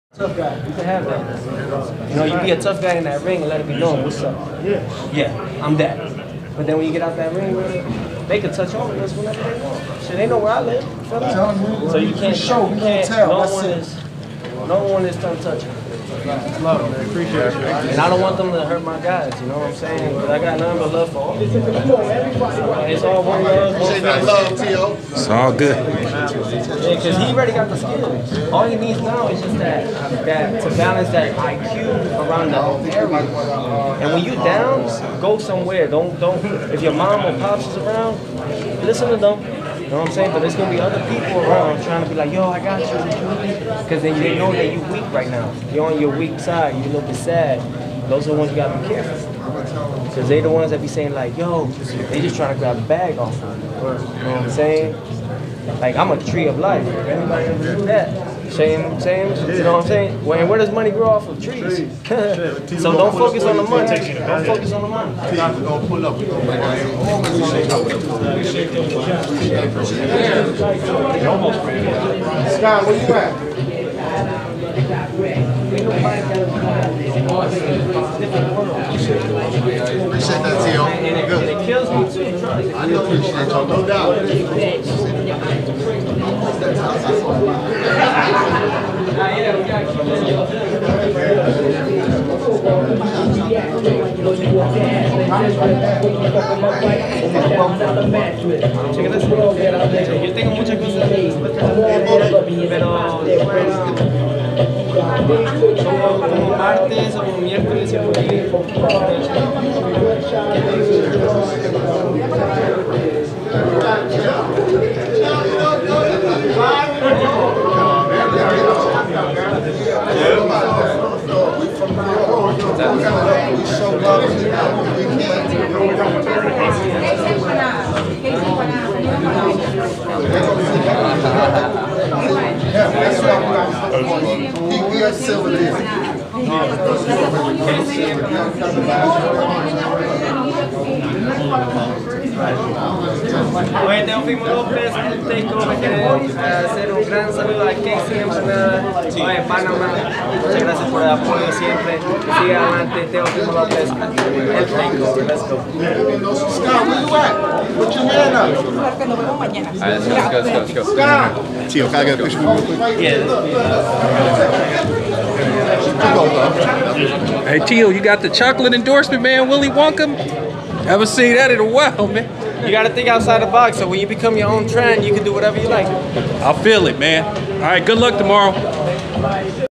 Teofimo and the chocolate bar
out of curiosity I had to ask what was up with him eating the chocolate bar after the weigh-in